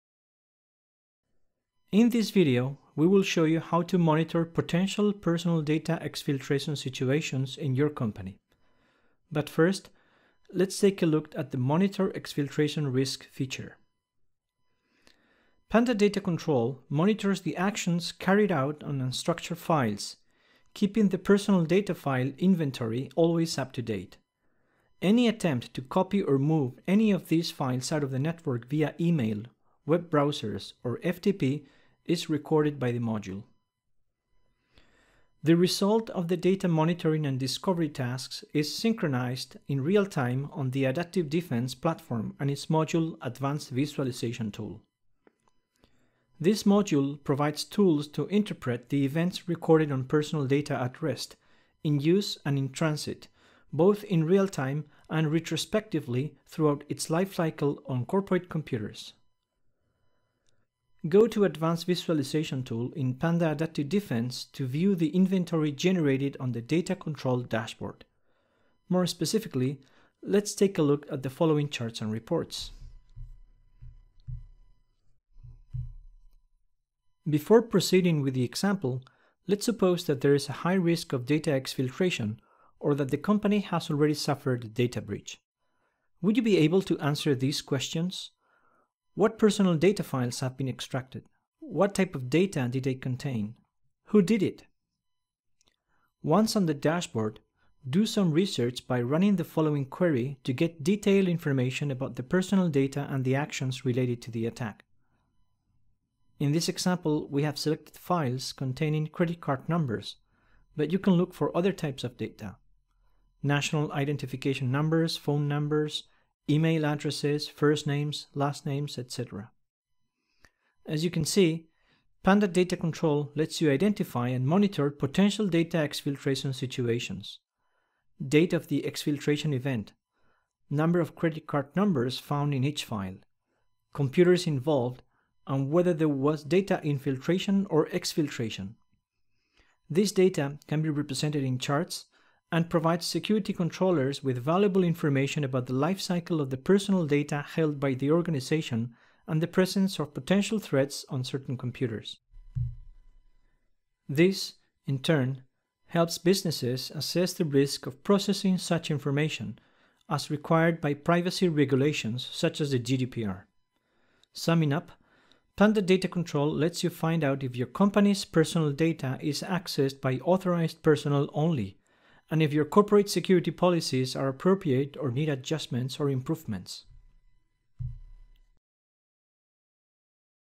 Potential Exfiltration Monitoring: Panda Data Control
Data Monitoring: Panda Data Control monitors the various types of operations performed on unstructured files (data in use), while keeping the personal data file inventory fully up to date. Any attempt to copy or move any of these files out of the network via email, Web browsers or FTP (data in motion) is recorded by the module.